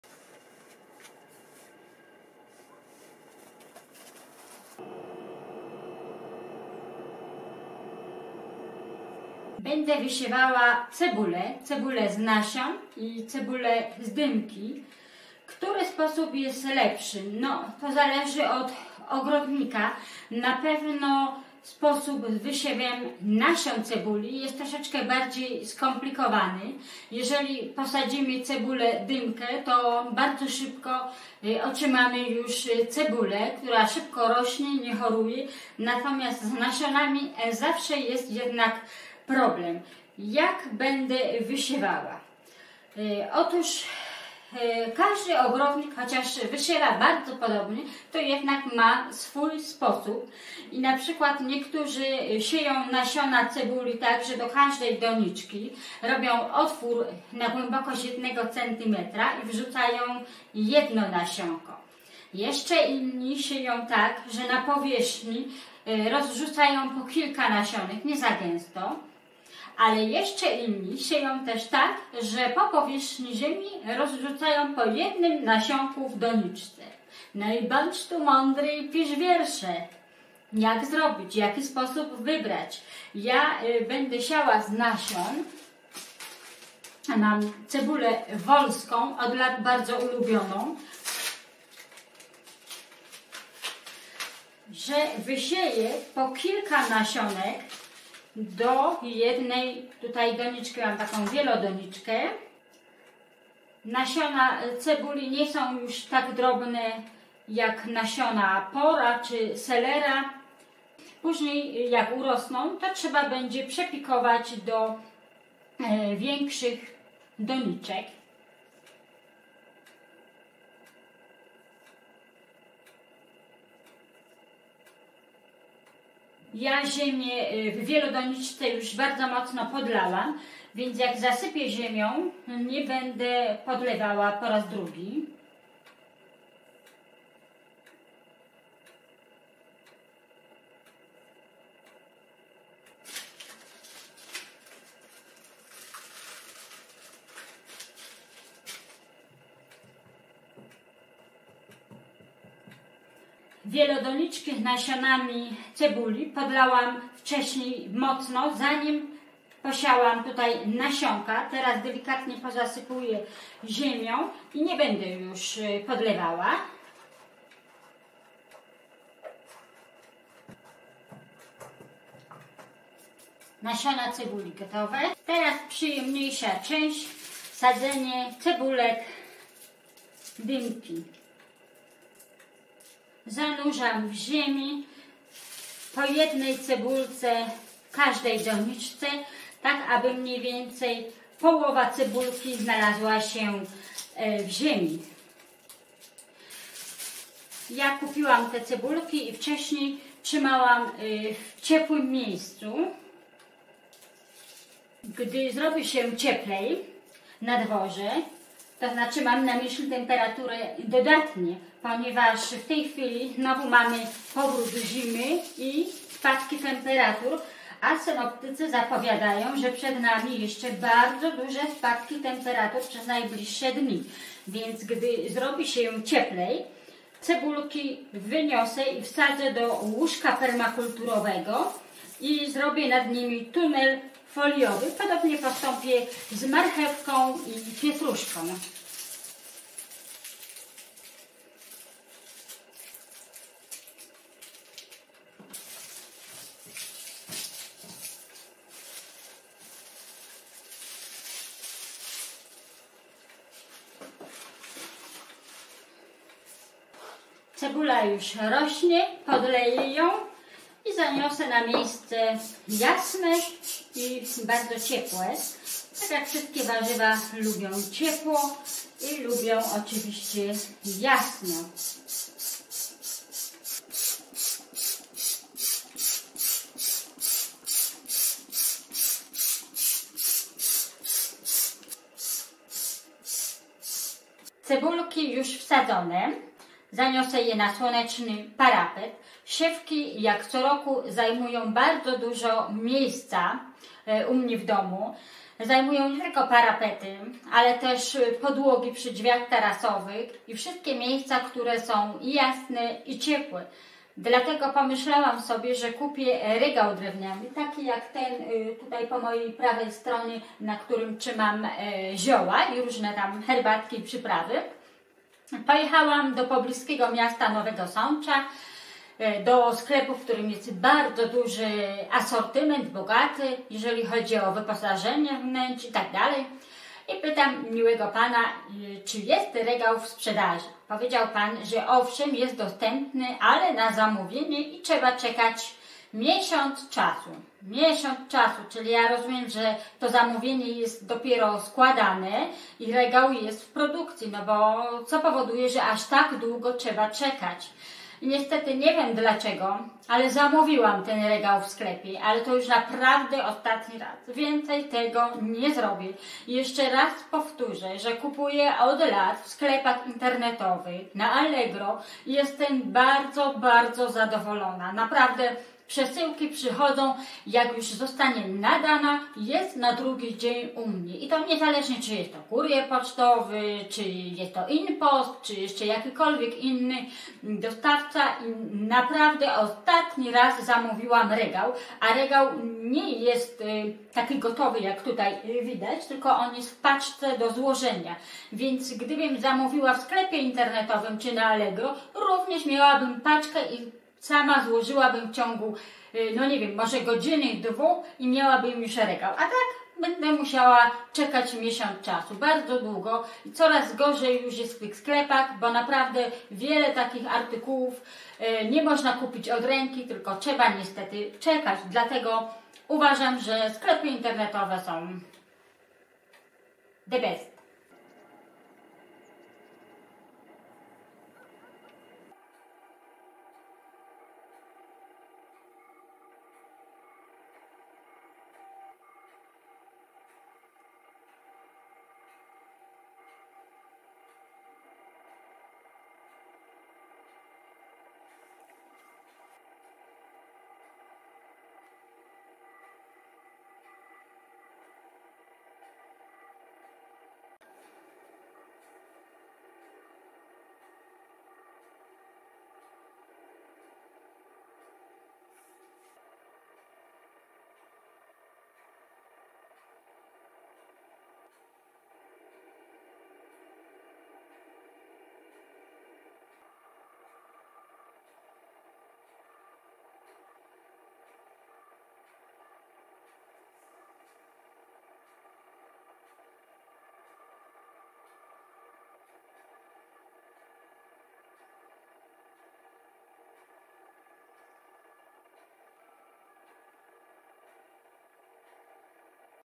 Siew cebuli z nasion i dymki. Uprawa cebuli, rozsada cebuli.
Cebula to warzywo bez którego trudno wyobrazić sobie potrawy w kuchni.
Bardzo serdecznie dziękuję za subskrypcje i liczne polubienia mojego kanału:)